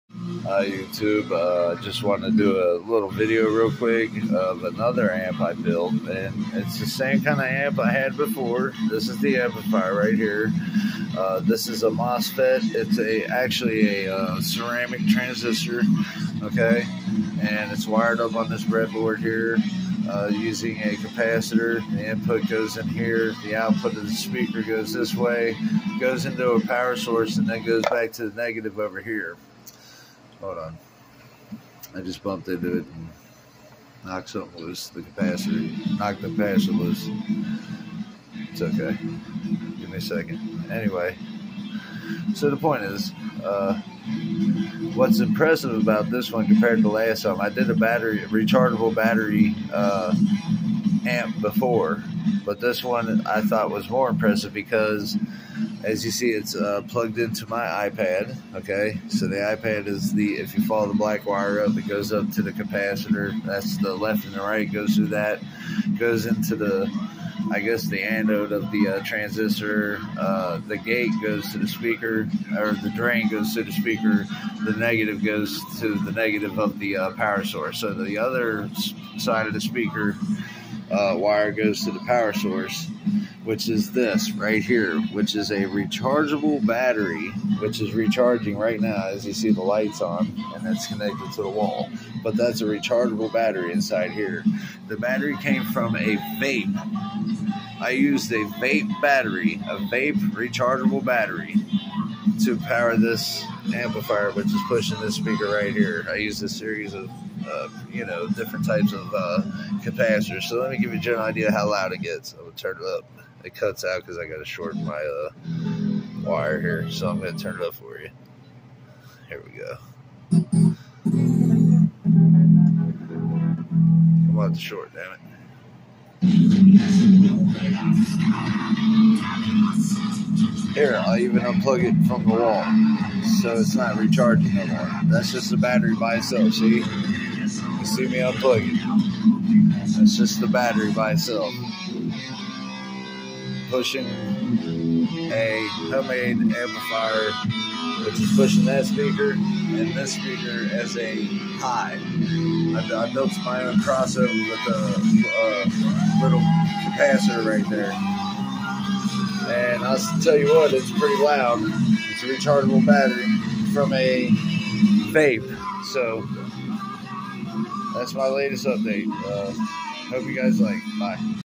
I rebuilt a phone MOSFET amp that used a rechargeable vape battery
phone amp pushed by rechargeable vape batteries